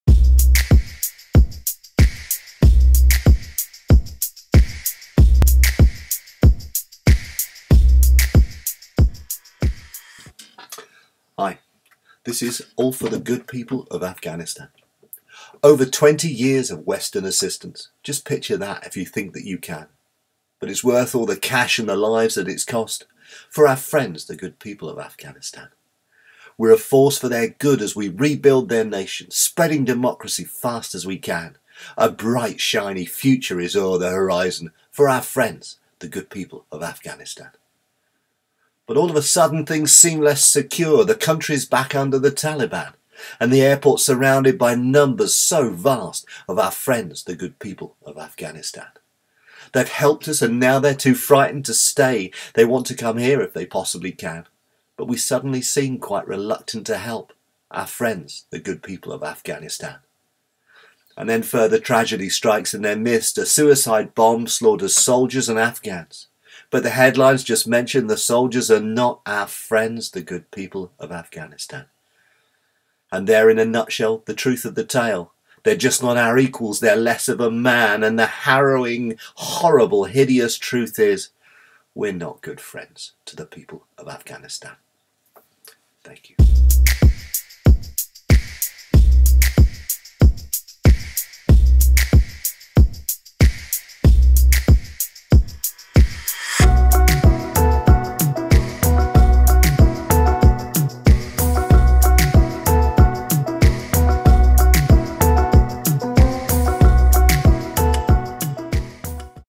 LT006 - All for the good people of Afghanistan
A short reflection on the recent withdrawal of British troops from Afghanistan, particularly in light of one specific piece of reporting, and just what that says about us.